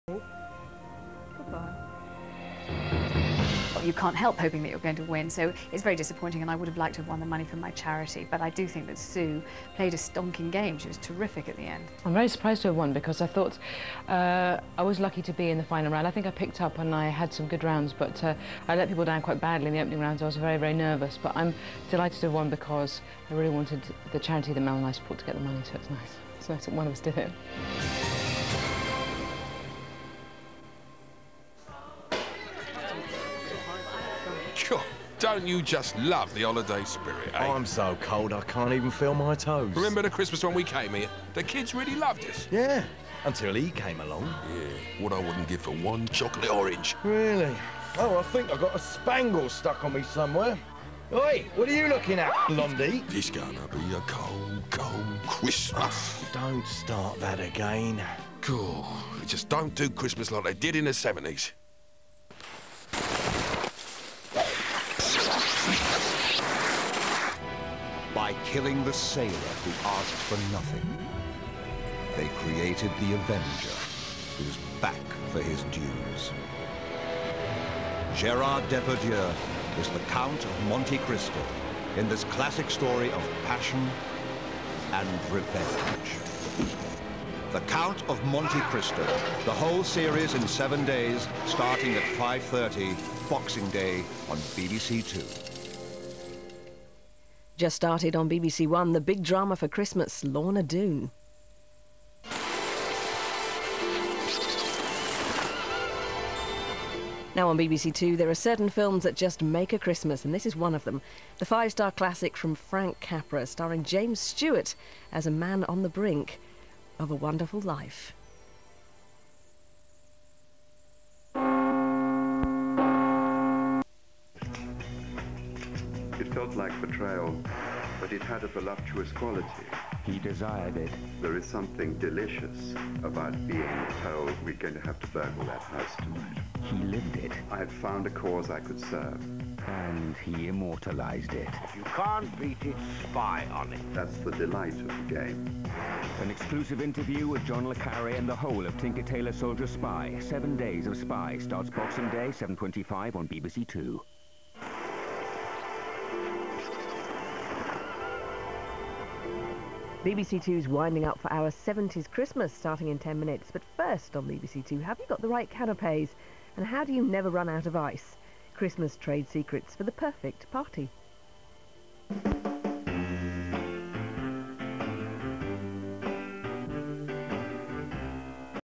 BBC Two Scotland - Continuity - (25th December 2000)
Merry Christmas, you lot! As a gift from me to you, I am supplying you with a lot of Christmas continuities throughout the day. Starting with some footage from the now-defunct Scottish feed of BBC Two, recorded in 2000.